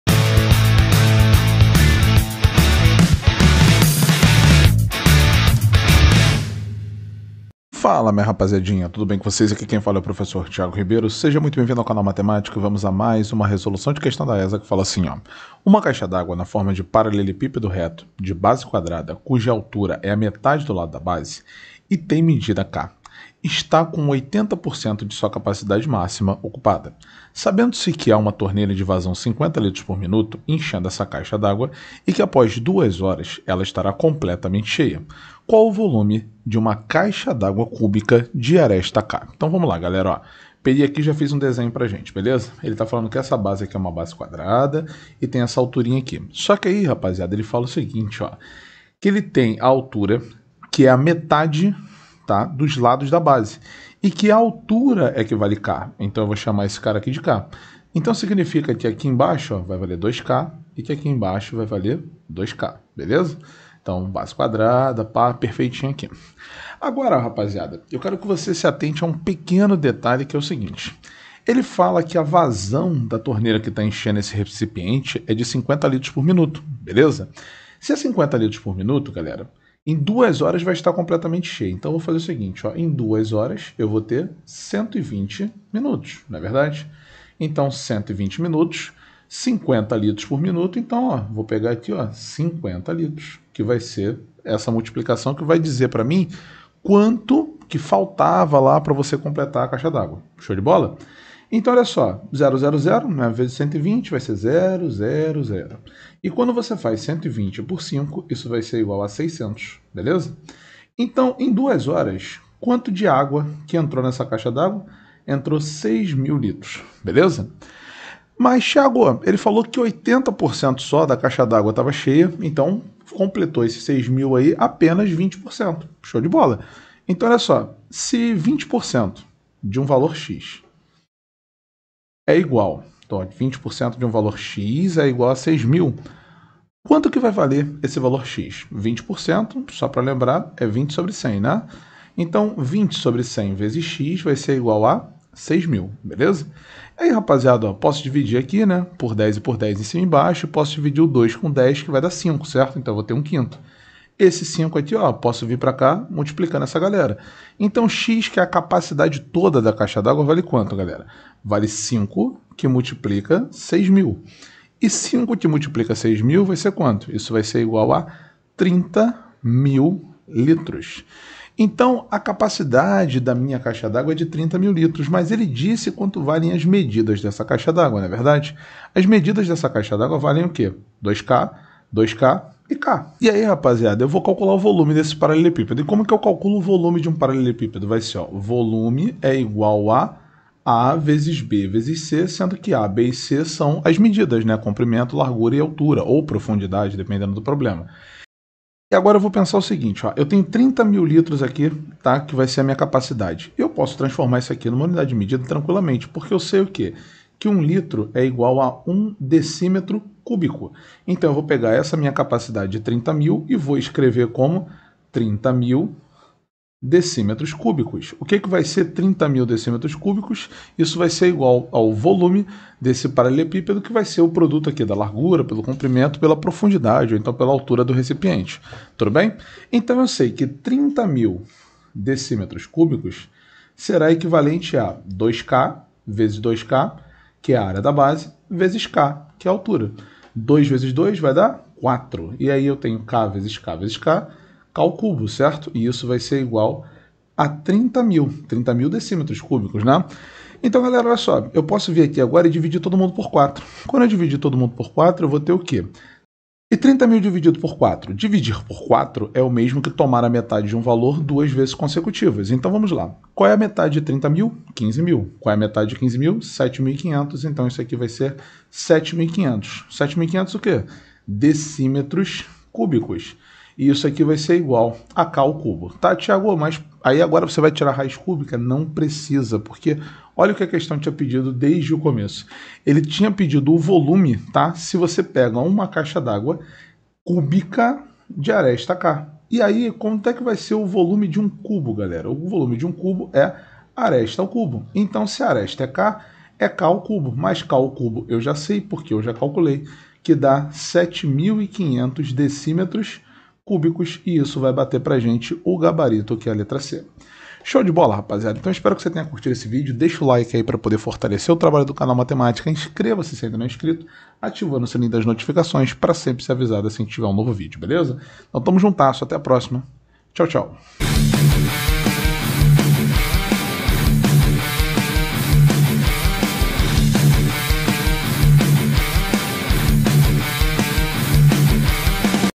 ESA | Uma caixa d’água, na forma de paralelepípedo reto de base quadrada, cuja altura é metade do lA
(ESA) Uma caixa d’água, na forma de paralelepípedo reto de base quadrada, cuja altura é metade do lado da base e tem medida k, está com 80% da sua capacidade máxima ocupada. Sabendo-se que há uma torneira de vazão 50L/min enchendo essa caixa d’água e que após 2h ela estará completamente cheia, qual o volume de uma caixa d’água cúbica de aresta k?

a) 7500 ml.
b) 6000 L.
c) 7500 dm³.
d) 5000 ml.
e) 6000 cm³.